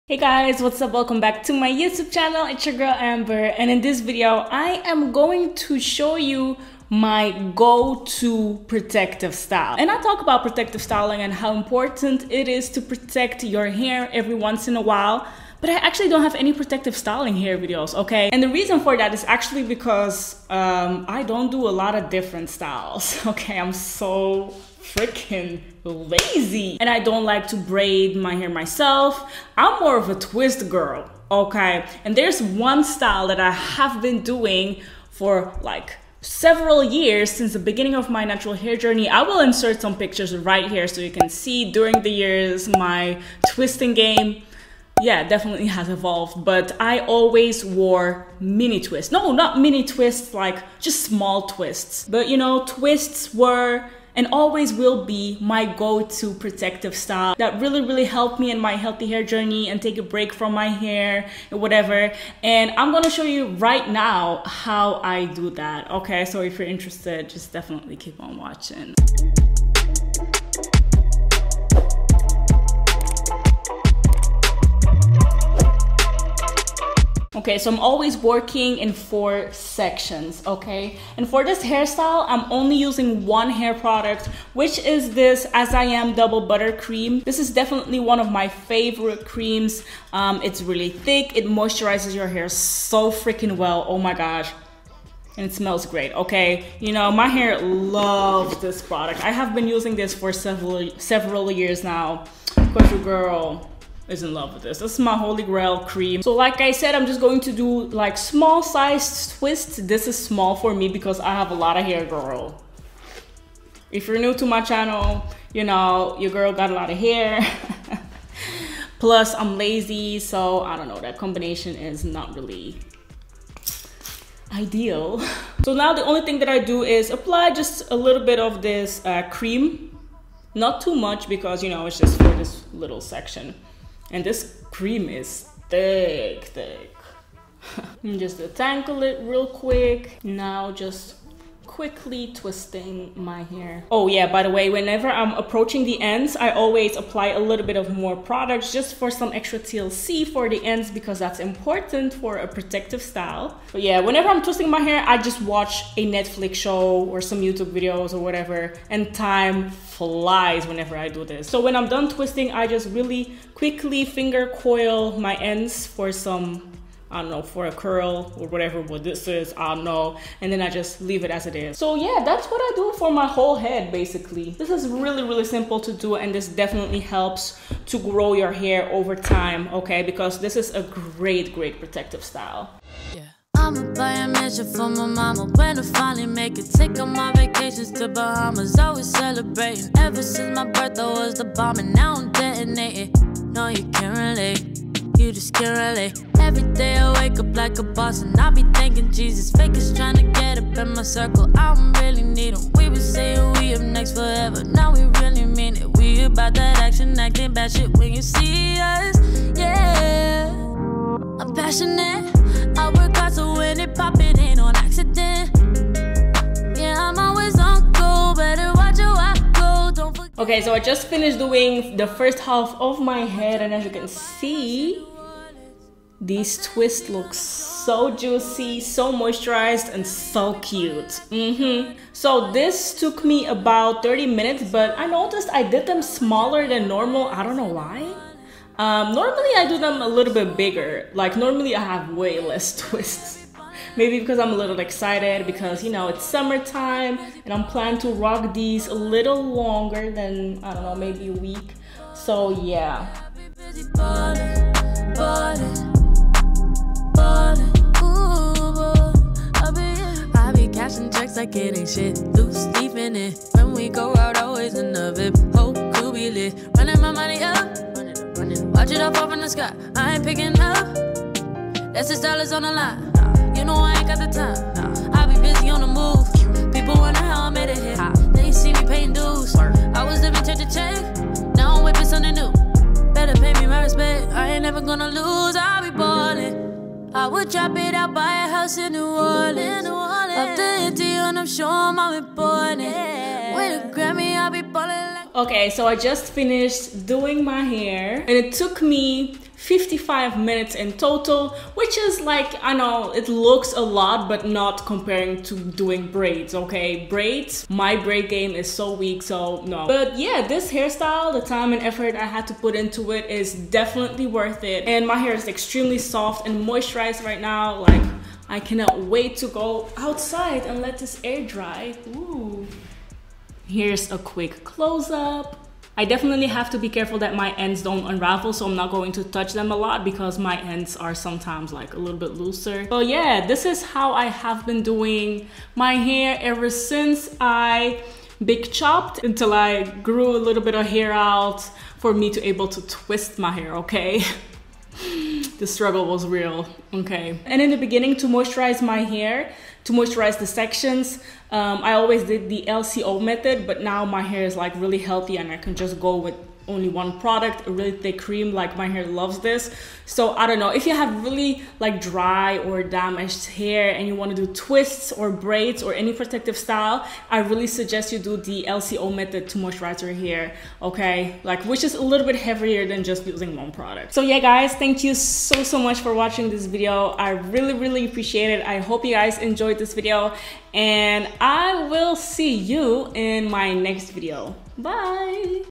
The BEST Protective Style For Natural Hair Growth
Hey loves! In this video I'm showing you my go to protective style (mini/small twists) that helped to grow out healthy natural hair. I have been doing this protective style for yearssss and it helped me so much!

FAQ's
- Ethnicity: Belgian and Ghanaian
- I live in Belgium, Europe
- I have an accent because English is not my first language, Dutch is.